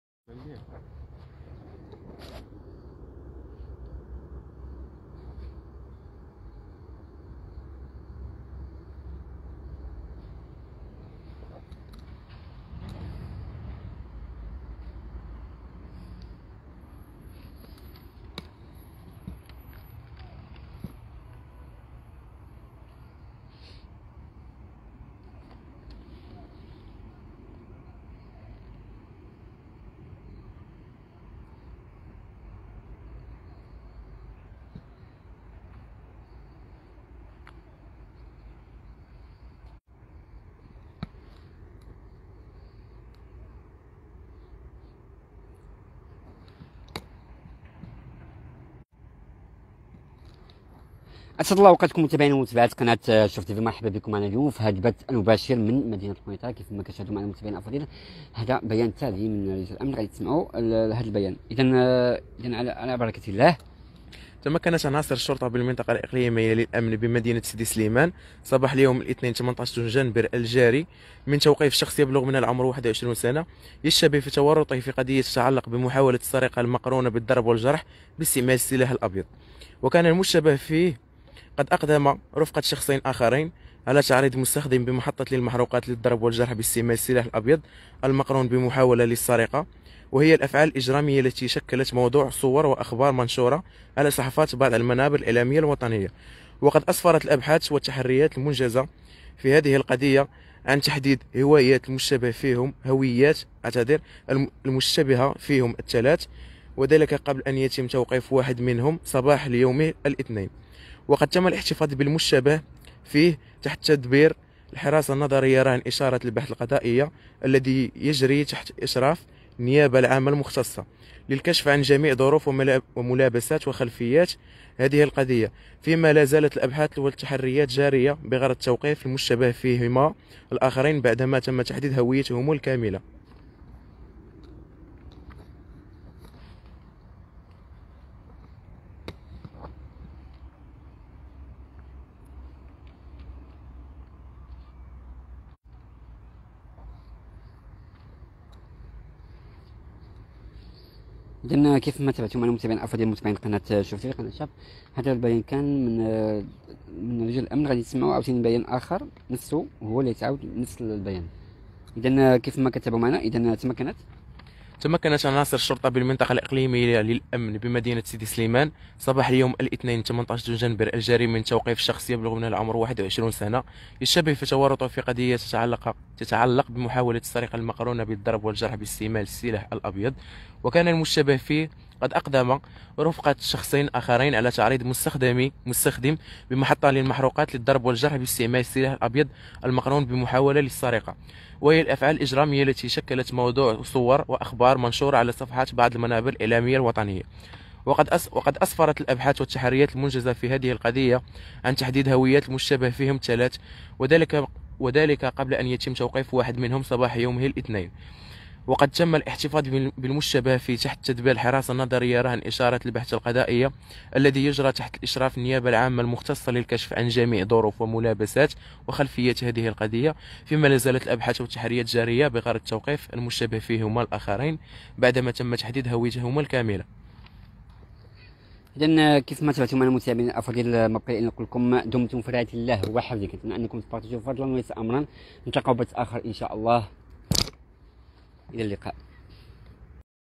أمن سيدي سليمان يوقف يشتبه شابا تورط في قضية تتعلق بمحاولة السرقة المقرونة بالضرب والجرح
مباشرة من القنيطرة: أمن سيدي سليمان يوقف يشتبه شابا تورط في قضية تتعلق بمحاولة السرقة المقرونة بالضرب والجرح باستعمال السلاح الأبيض